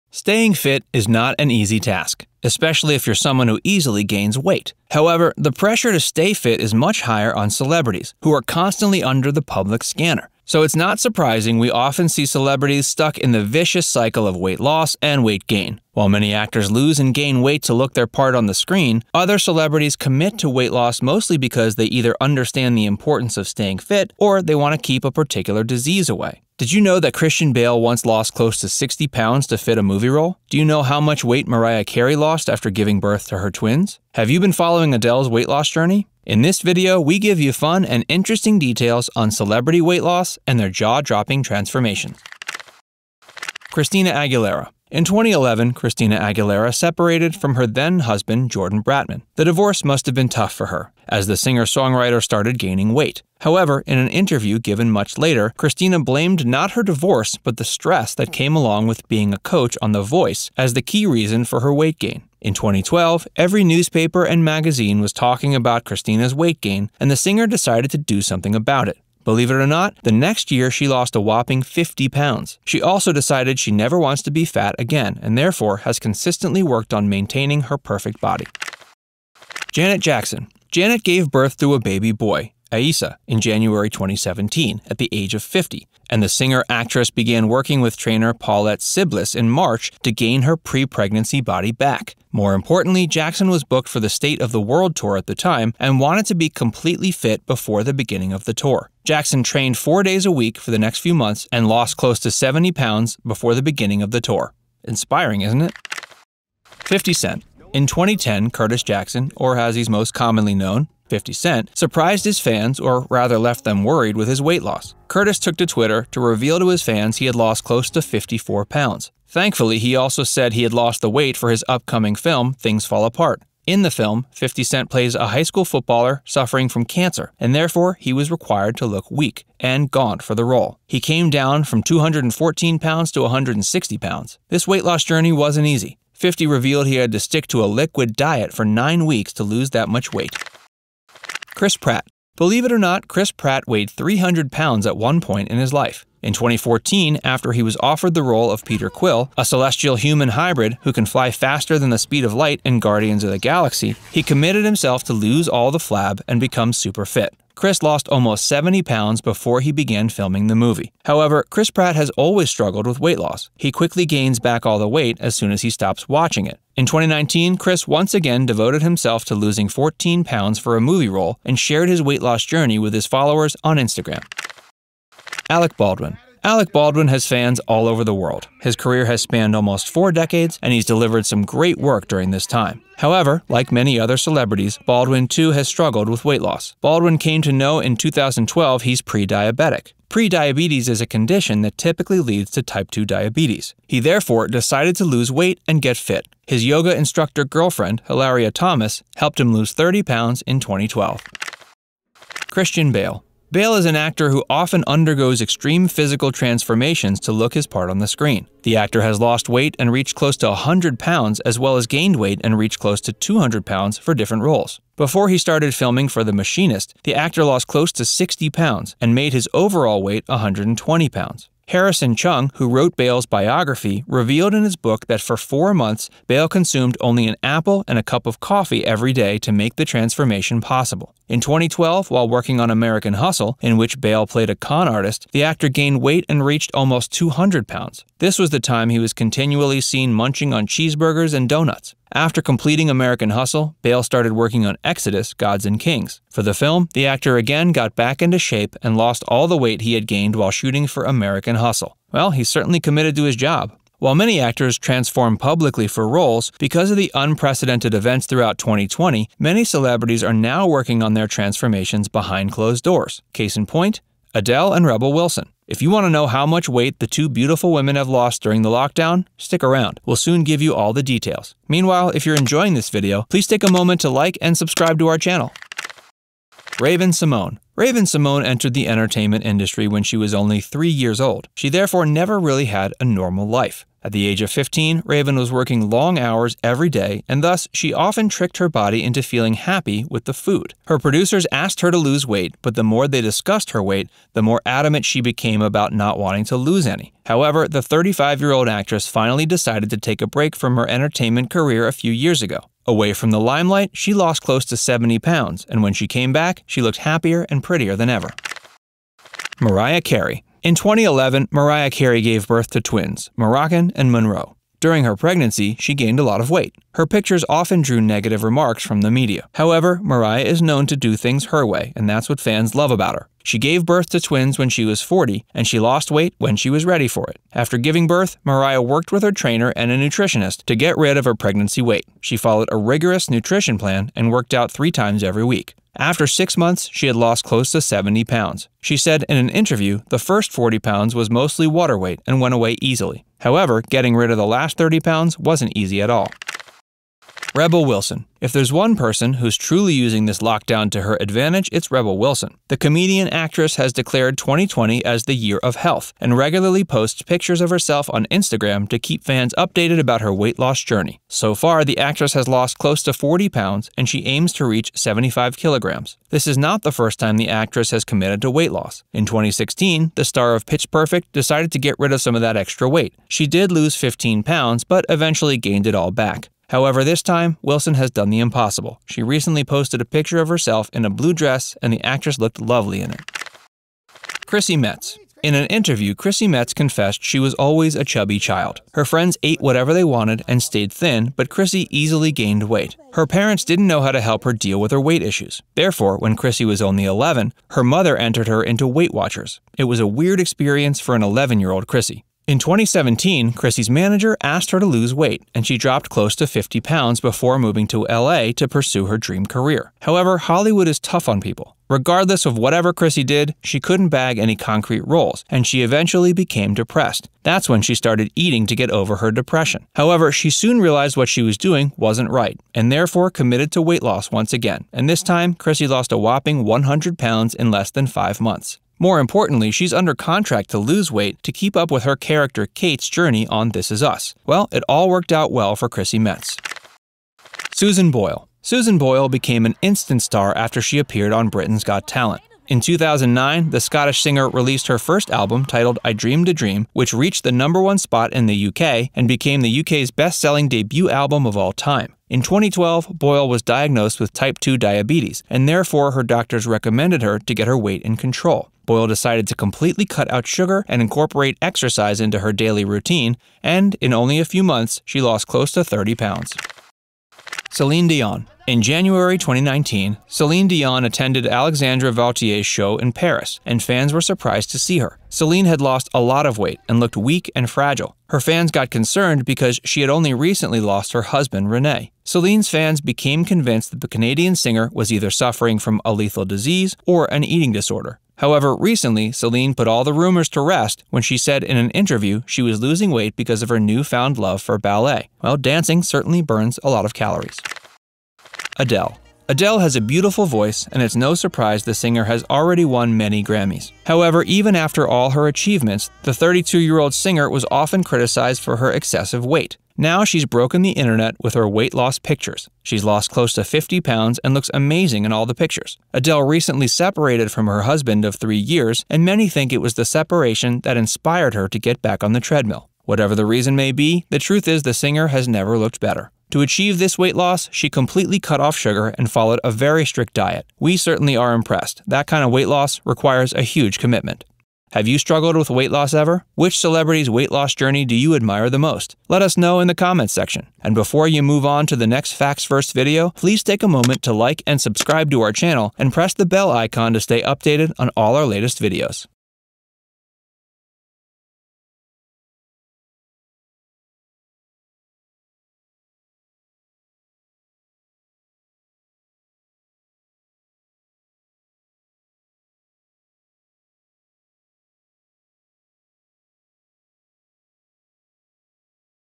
Celebrities Reveal Their Bodies After Weight Loss
Celebrities are always under the public radar and while they bask in the love of their millions of fans, they also often draw criticism. Like everything else in their life, their weight is often a topic of discussion. It's hard to imagine what this kind of criticism can do to a person's self-confidence. Therefore, we have decided to celebrate and appreciate some of the celebrities who dropped pounds with their hard work and commitment.